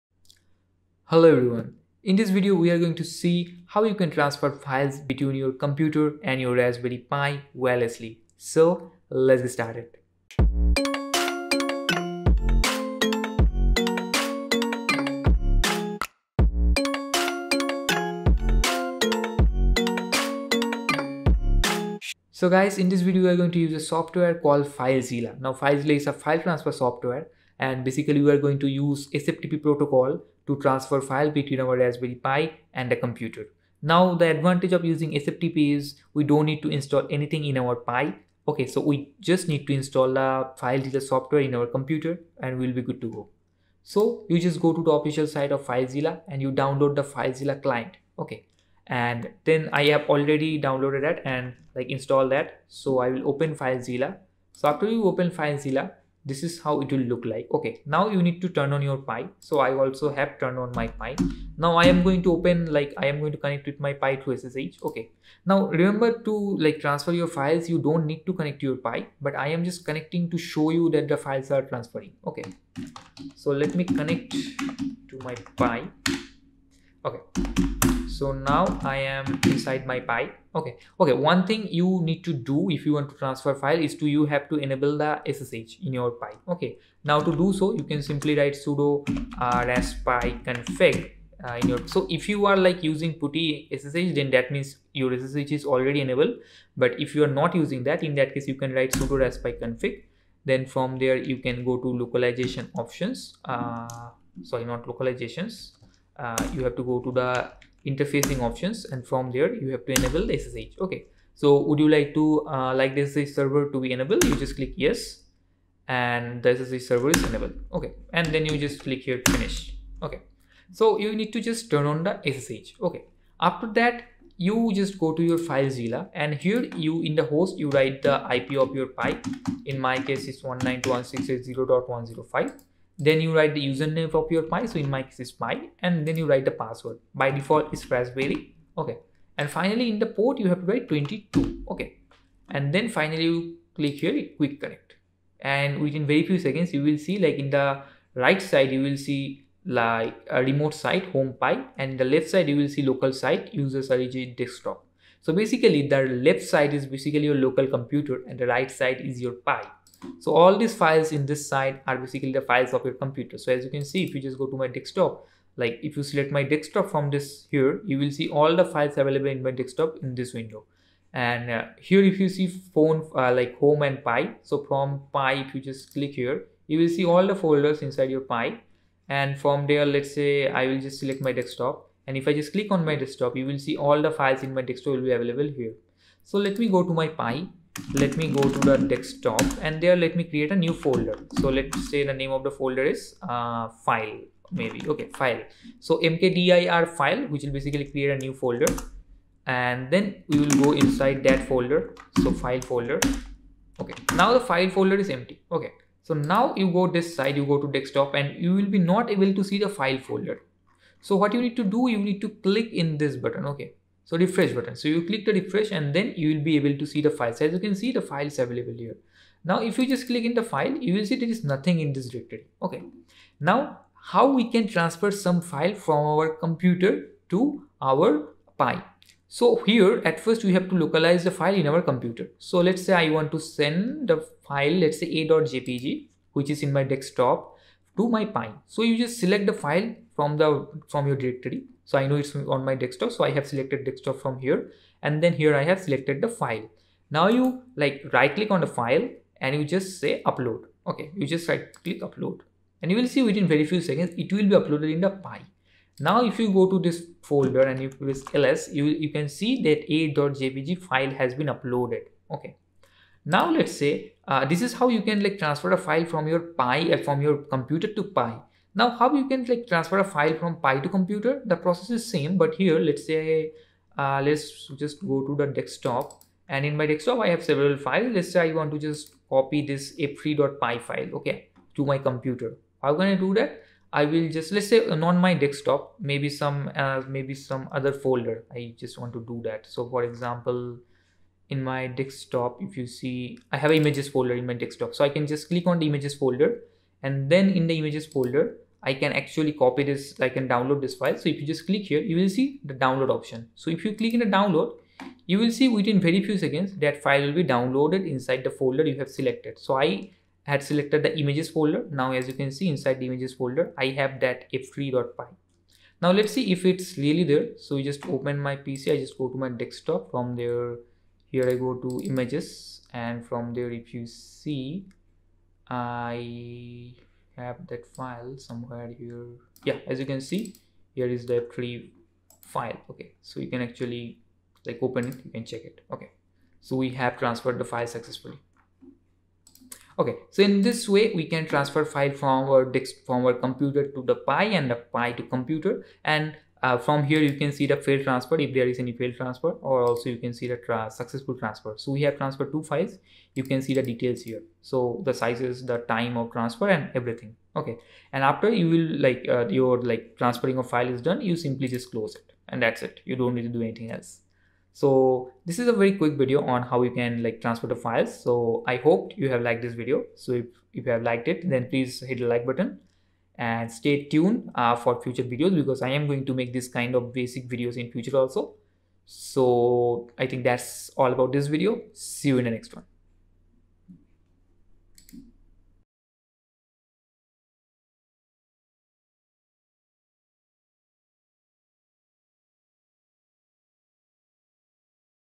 Transferring Files Between Pc and Raspberry Pi Wirelessly using FileZilla
In this video I will show you, how you can transfer files between your pc and Raspberry Pi using FileZIlla.